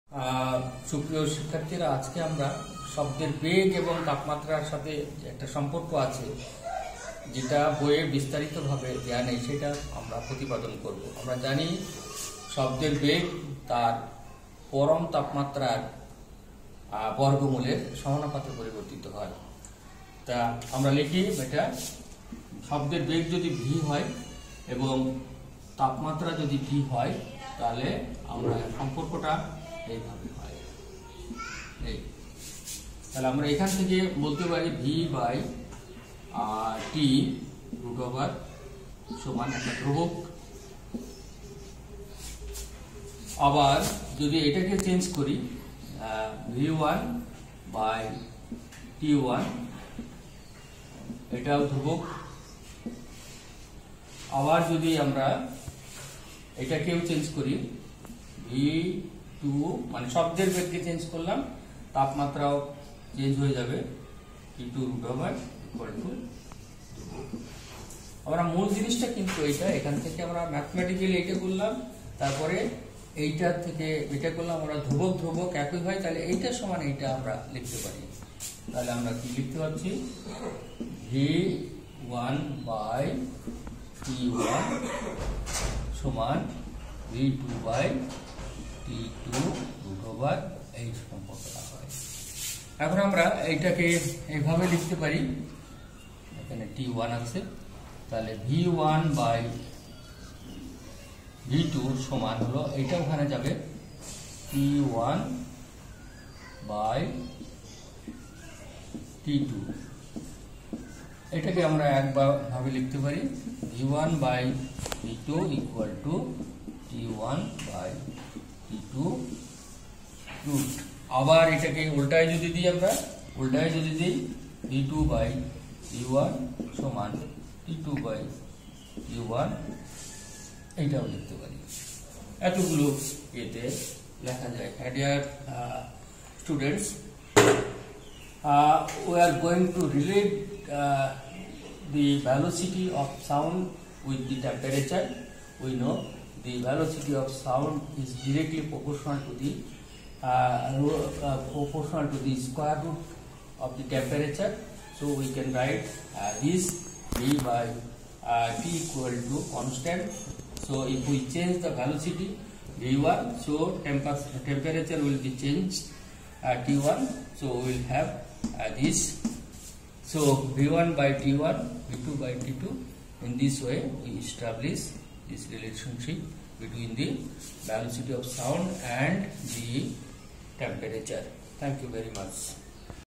শব্দের বেগ ও তাপমাত্রা Velocity of sound and temperature#sounds
To we will talk about  the velocity of  sound and the  temperature.